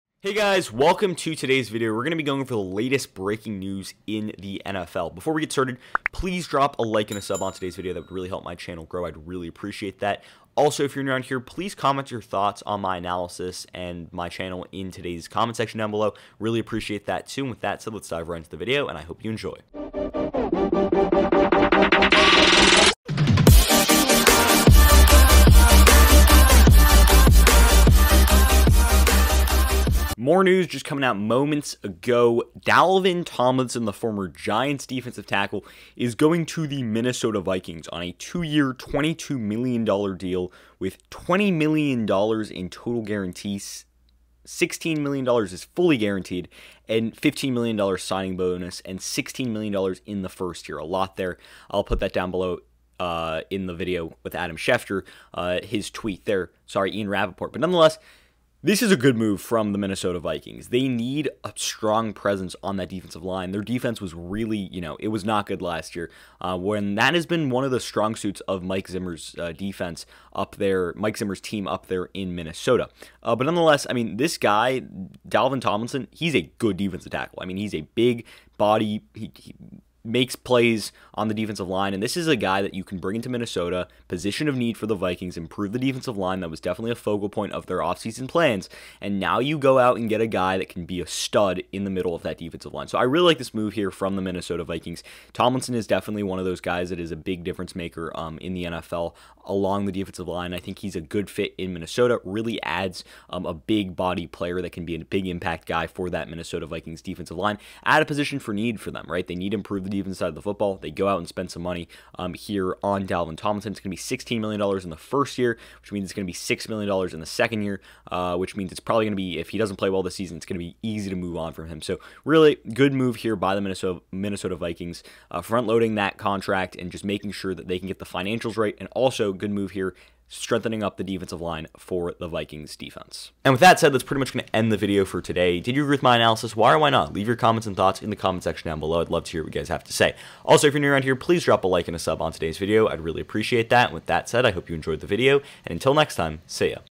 Breaking Down and Analyzing Dalvin Tomlinson Signing With the Minnesota Vikings
In today's video we'll be breaking down Dalvin Tomlinson signing with the Minnesota Vikings defense and what it means for the Vikings defense in 2021.

Photo by Al Bello/Getty Images